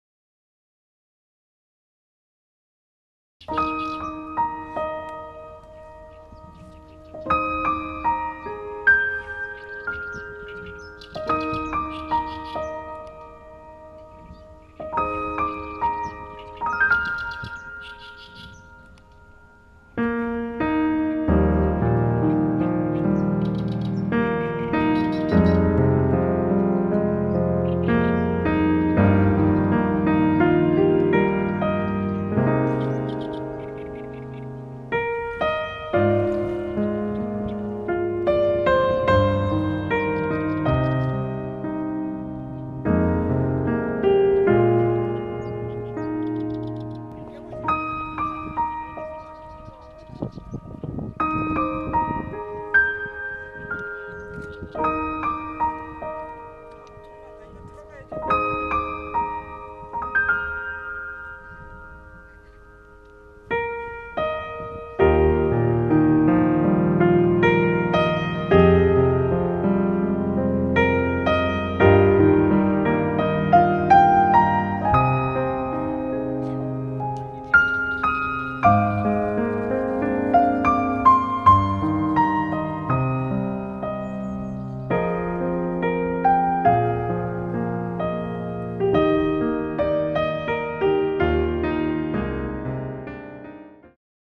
2016 물의정원 풍경 : 꽃양귀비
2016년 5월 물의정원을 가득 수놓은 꽃양귀비의 정취를 느껴 보세요.